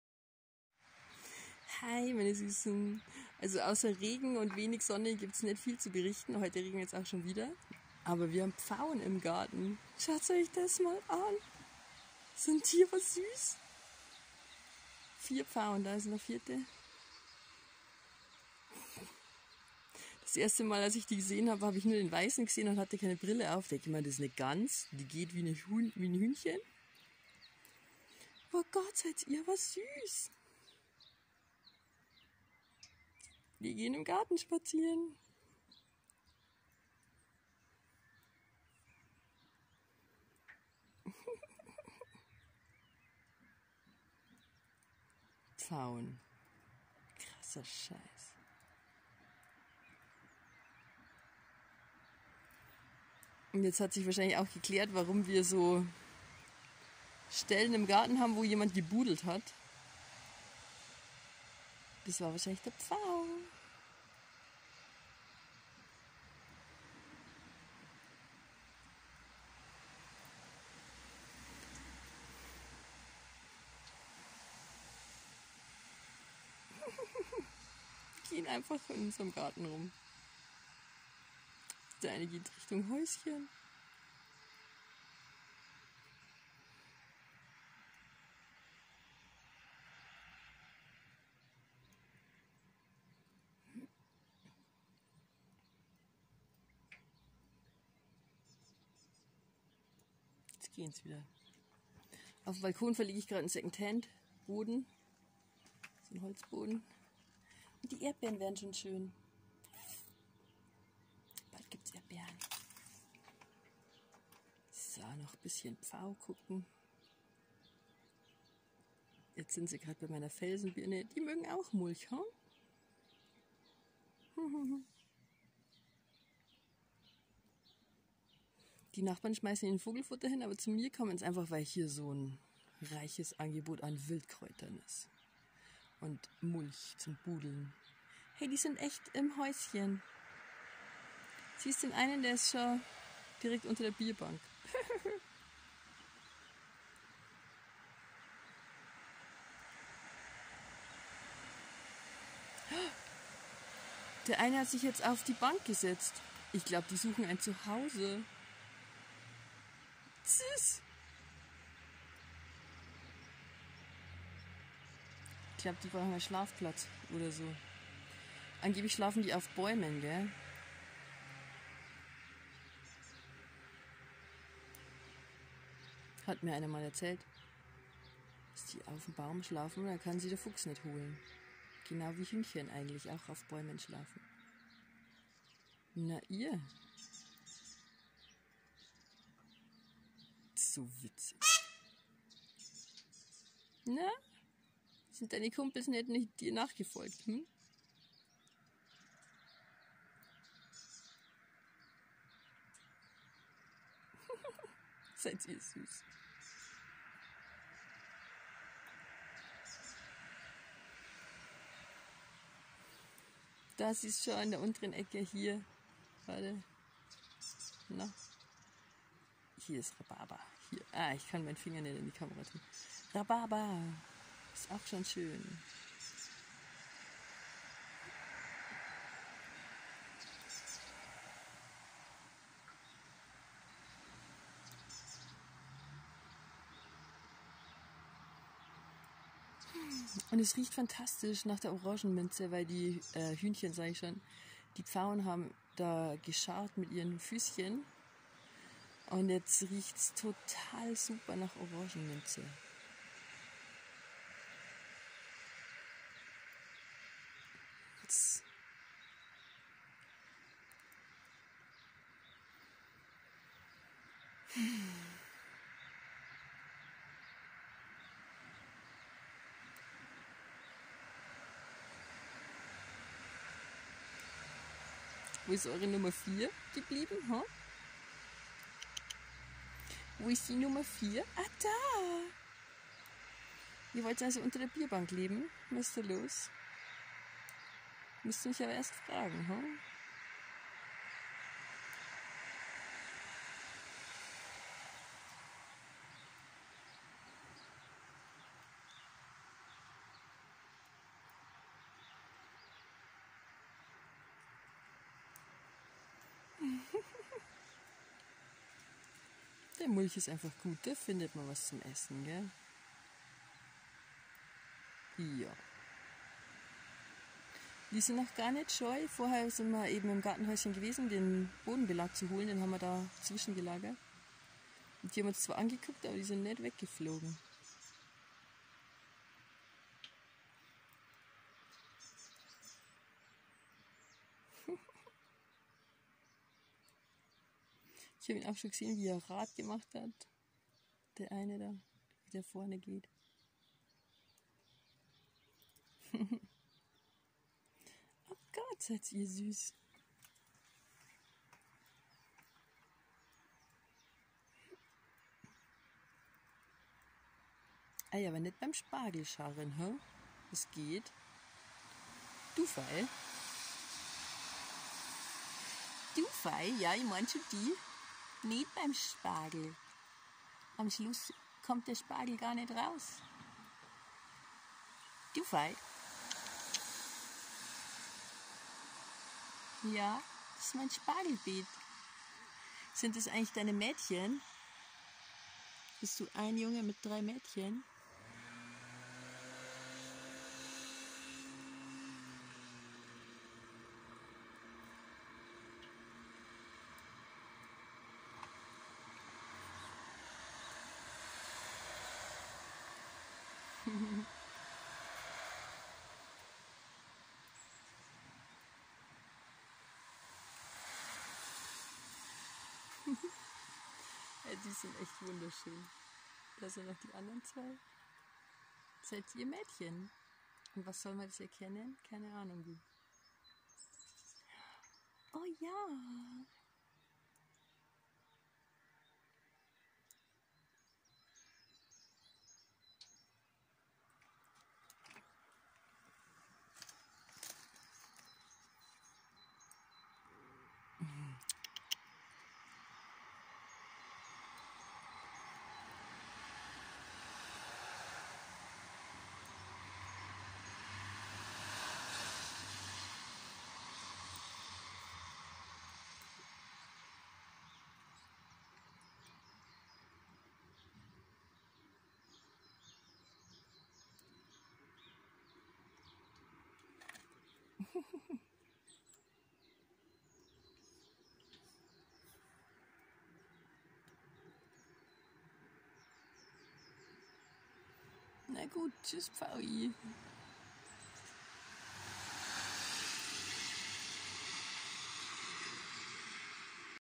Pfau im Garten! 🤩
4 Pfauen gehen in unserem Garten spazieren!